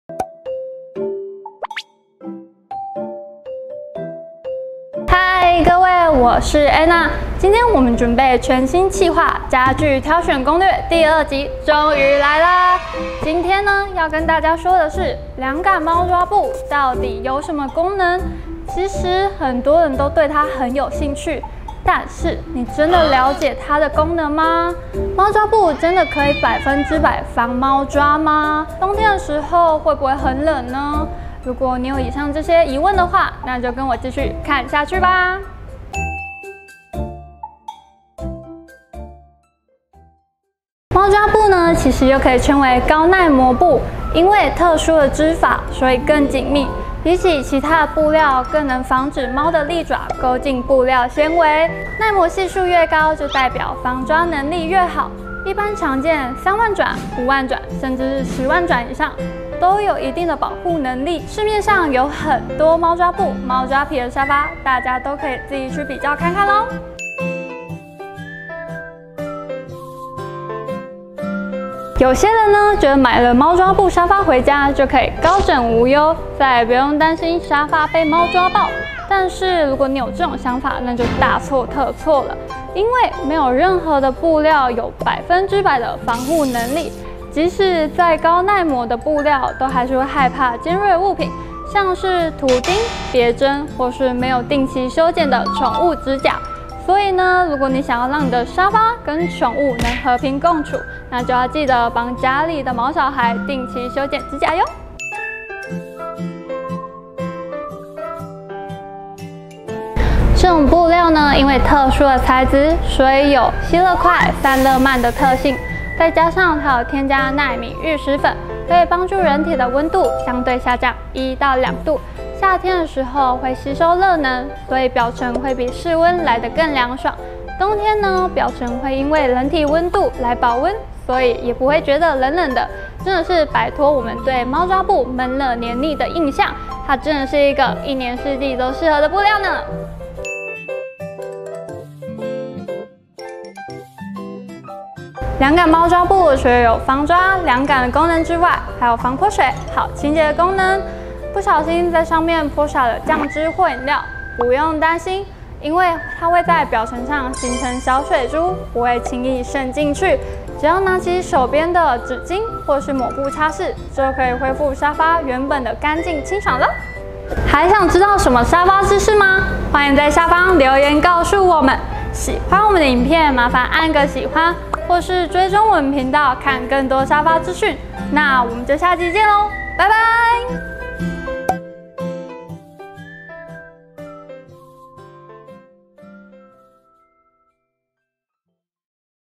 沙發選「涼感貓抓布」真的有那麼神❓買了貓抓布為什麼沙發還會破❓｜家具挑選攻略篇 ｜EP.02｜ MR.SOFA CASA
超人氣影片推薦給你💗